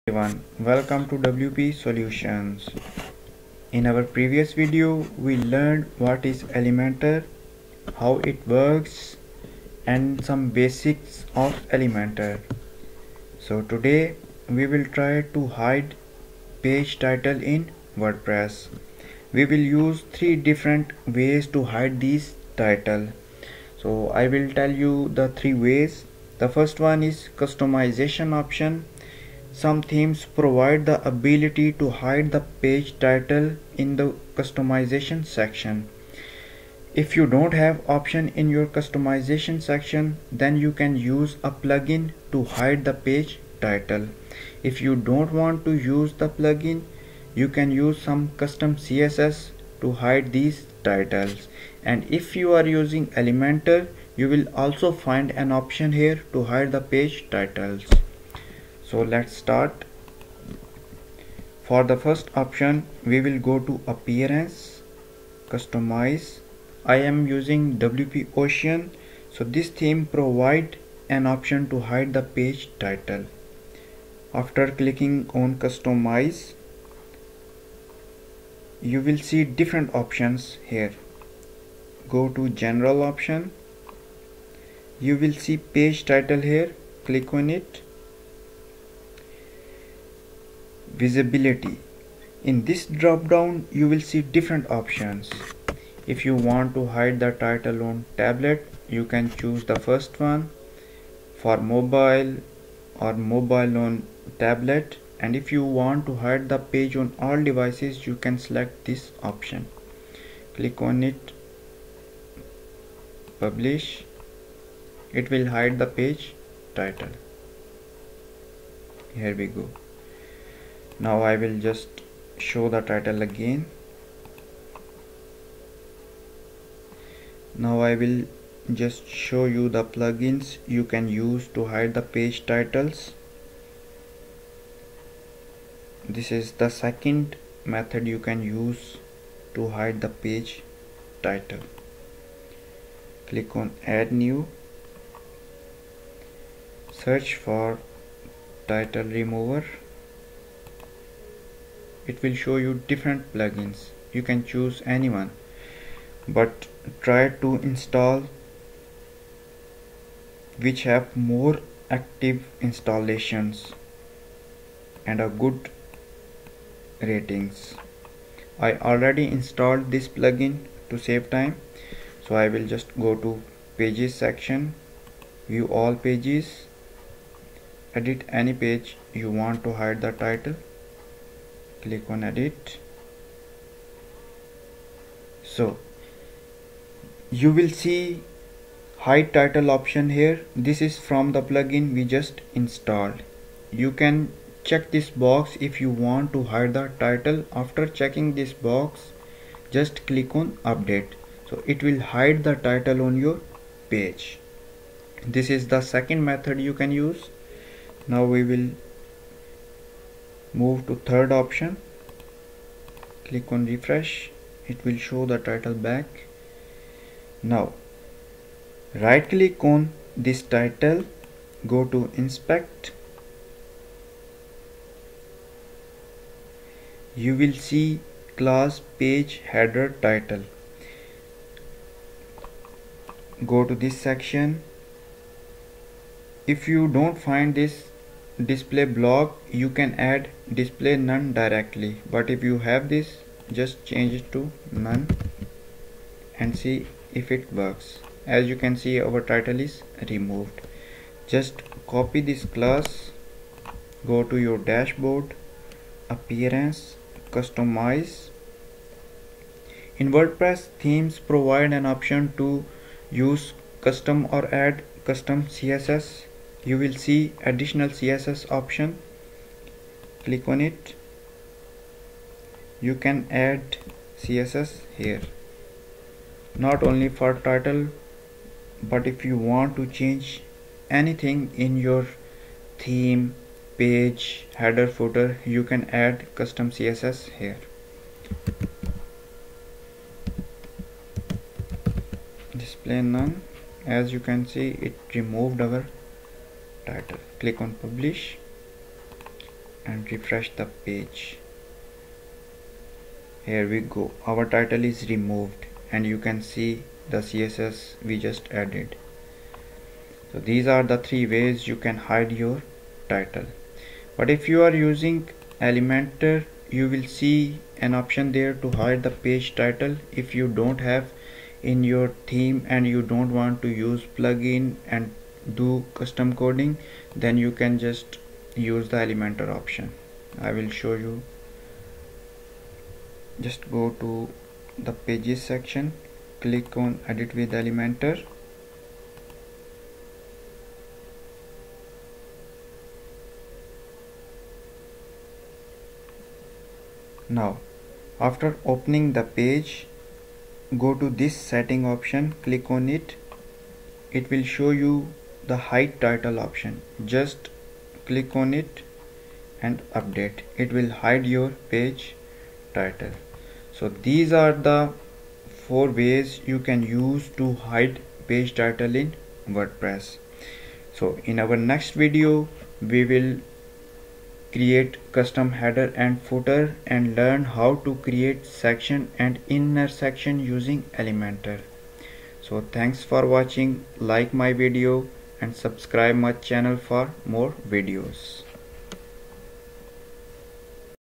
how to remove page title in wordpress | wordpress basics
In this video we will cover how to remove page title in wordpress. We will use 4 different and easy ways to hide page title. We will learn some wordpress basics in this video.